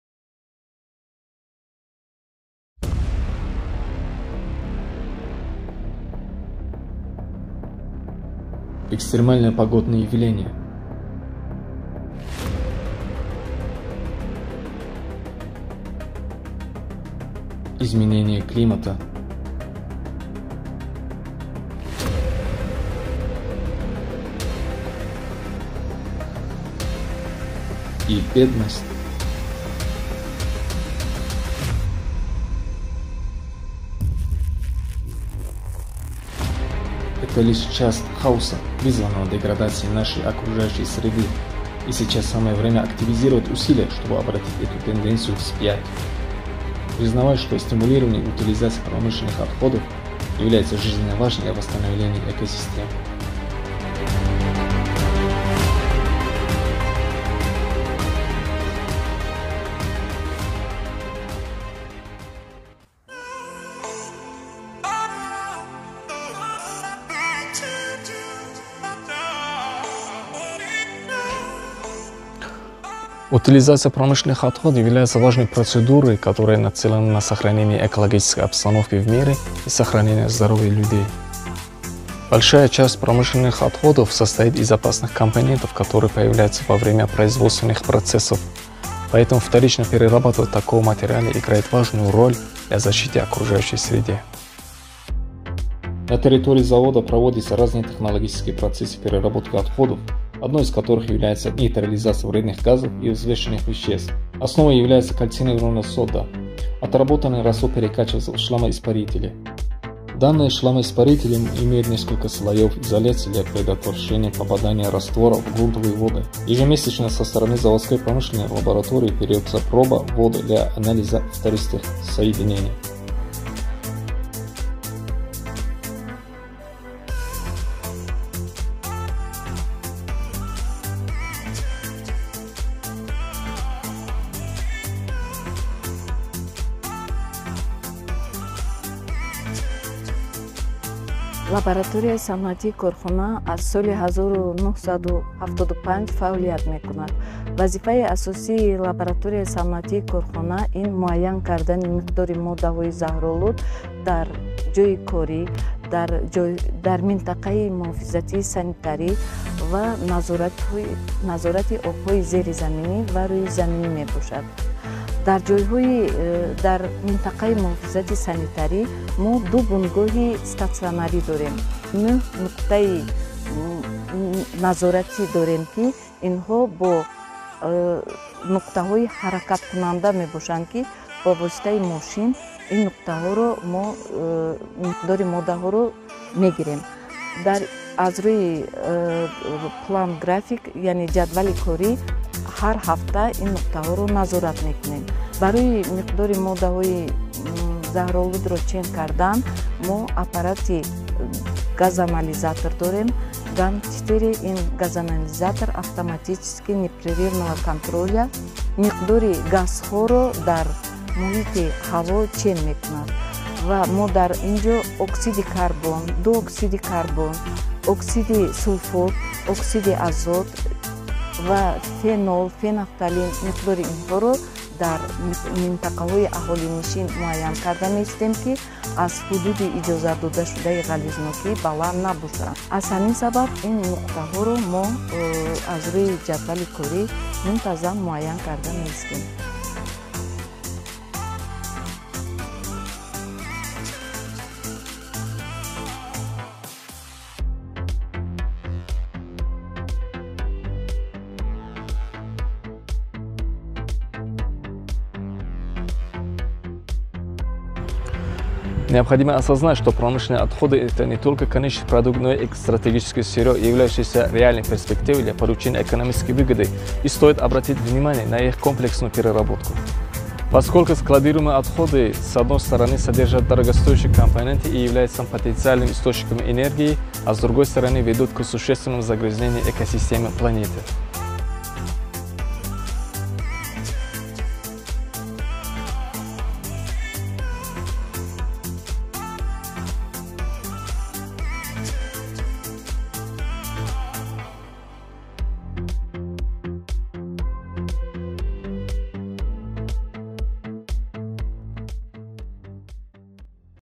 Промышленные отходы
Промышленные отходы -это вещества и материалы, которые несут угрозу экологии и здоровью человека. Как правильно переработать эти отходы?
Видеоролик подготовлен в рамках курсов Вечерки по видеографии и производству контента по проблемам экологии - нашим блогером из Турсунзаде Мавлончоном Каюмовым